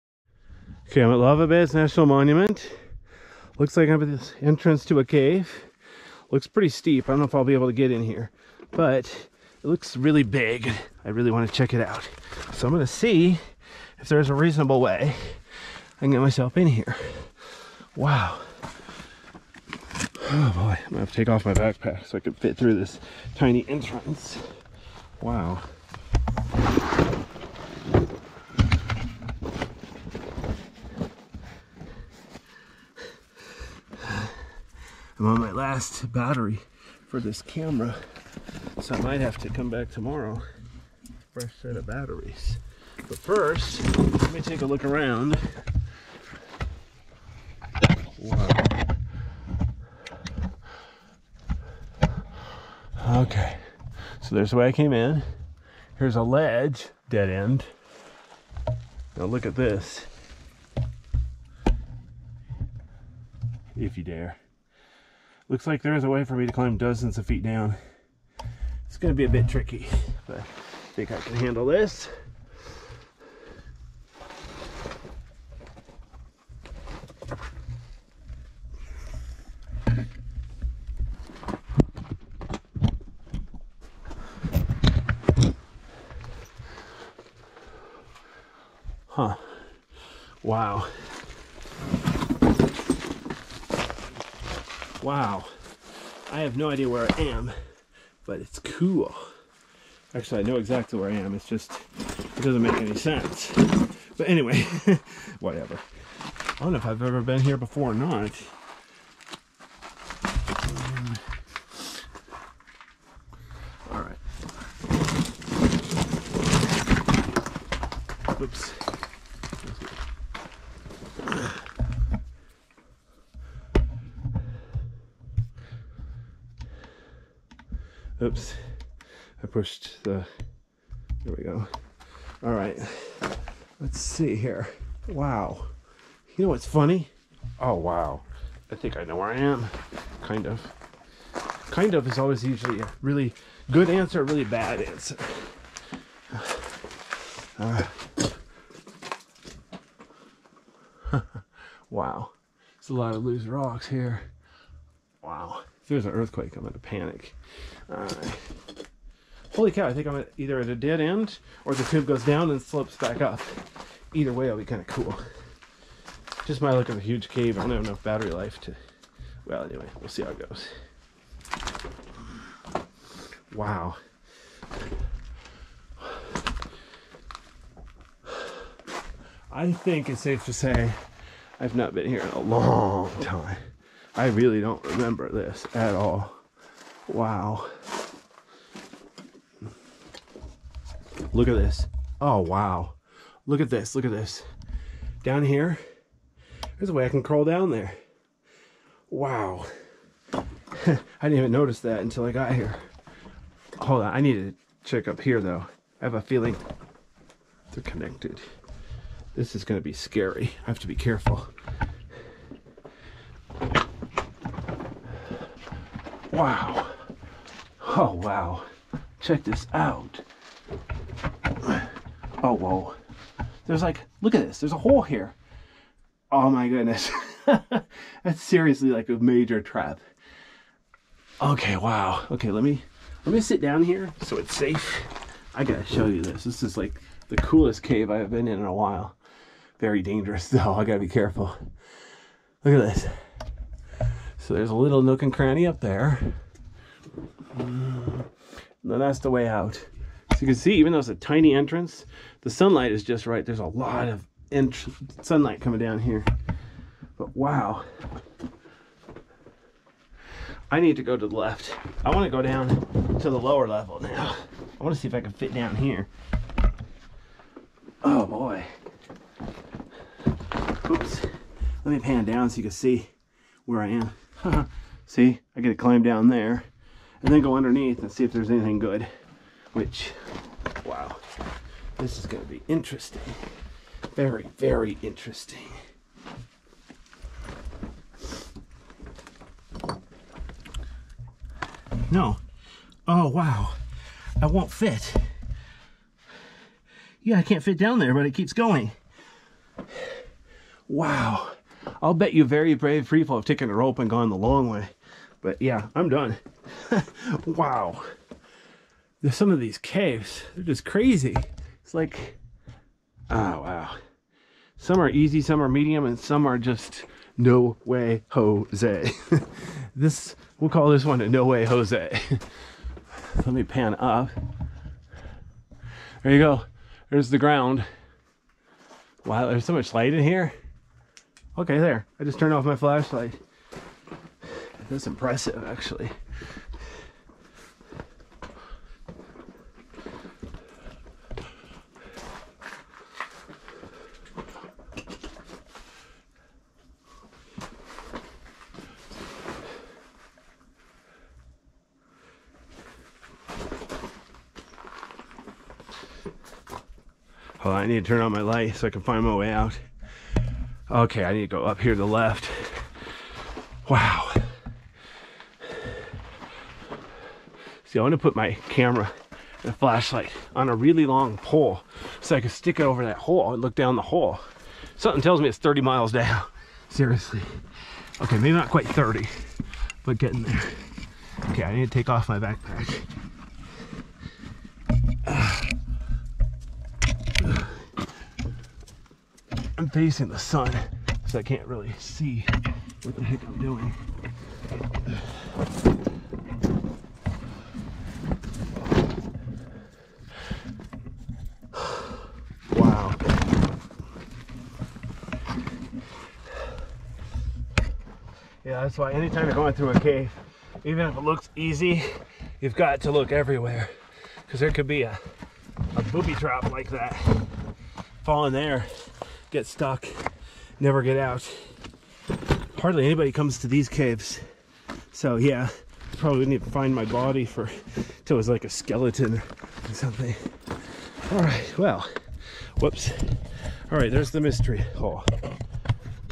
mystery cave, video 1, 5k 30fps 2880p30
This is a lava tube at Lava Beds National Monument in northern California.